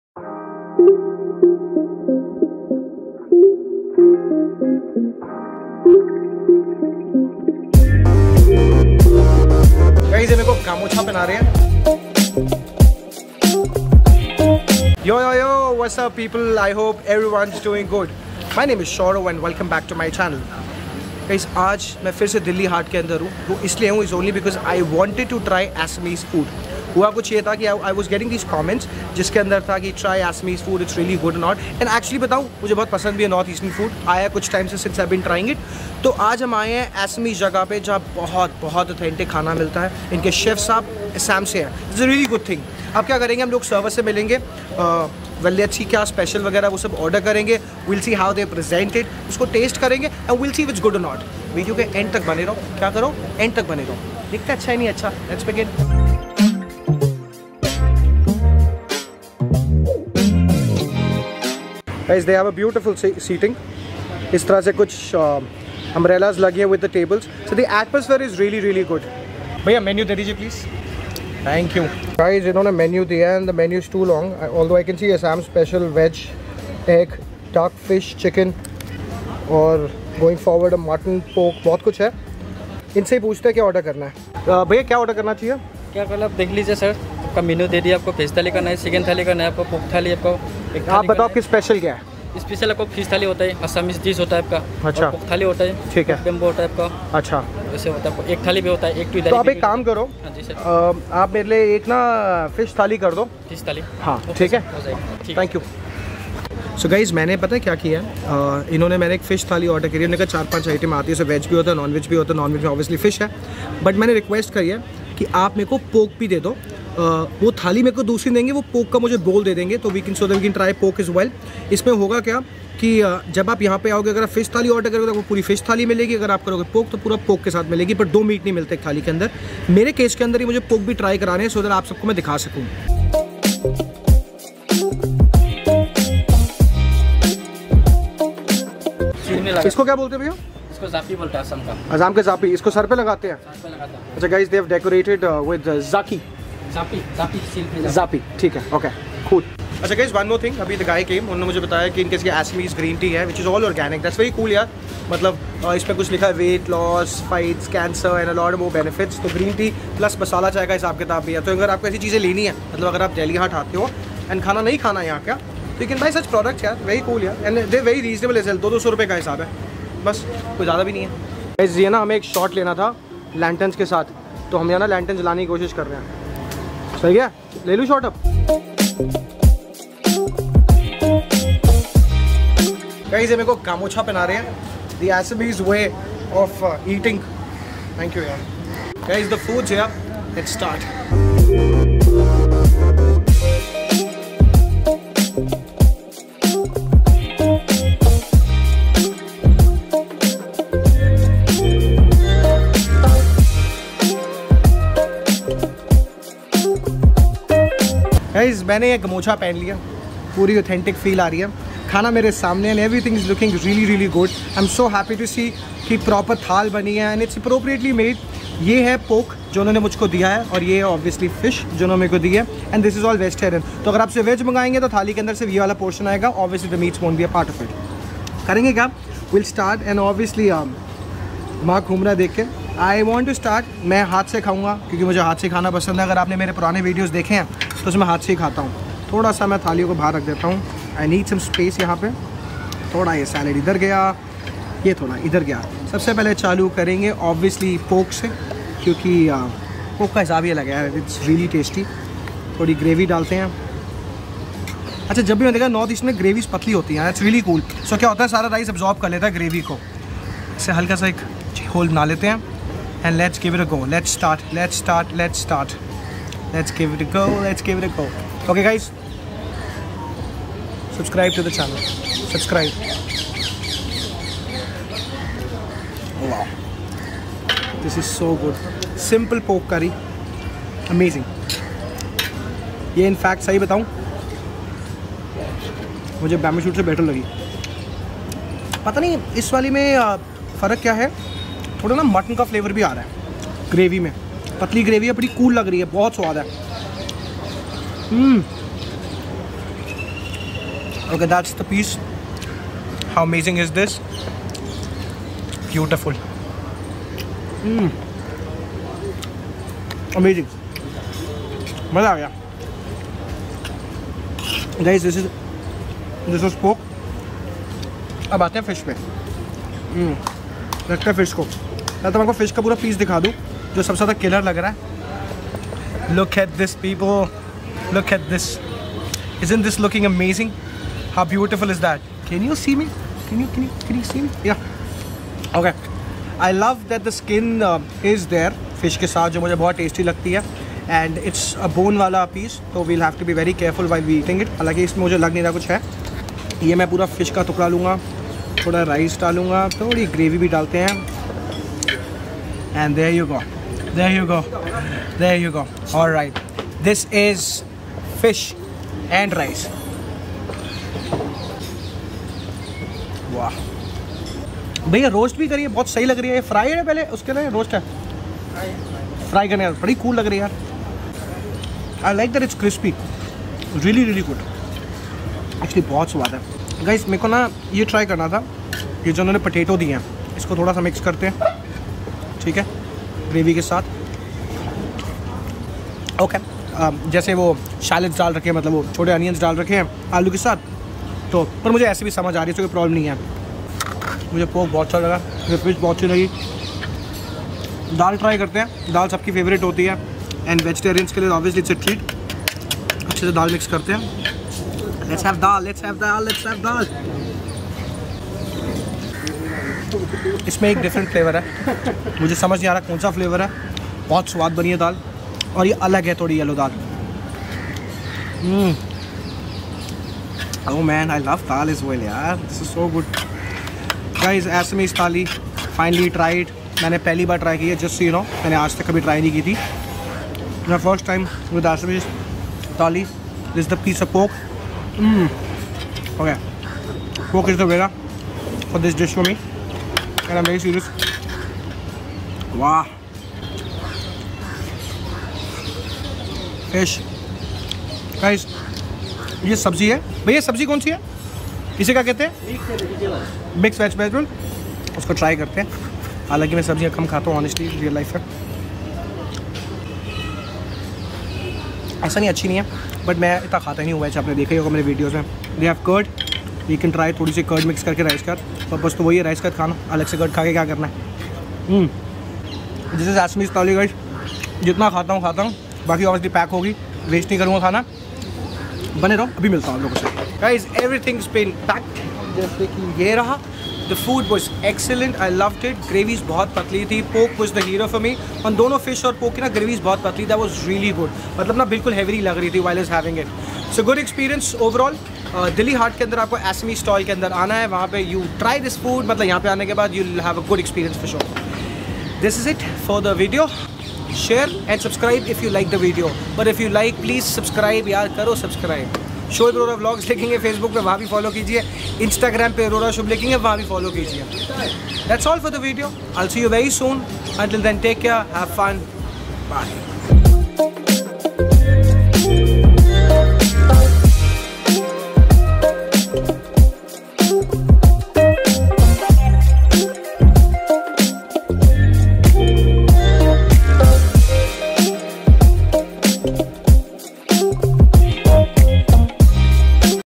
Traditional ASSAMESE THALI with Pork Curry, Fish Curry in Delhi, India
Hello Peeps,

I am back on trying North  Eastern food. This time it was the very famous Assamese thali. I loved the authentic way of presenting it. It was tasting so good yet different. North eastern food has never bee disappointing. The food was pocket friendly and worthy. I will recommend this place for a good experience. It is located in the heart of Delhi. Order for their authentic north eastern thali and you will indeed be happy.

Location: Dilli Hart, Delhi

Stall Name: Assam Food Stall

Shoaib Arora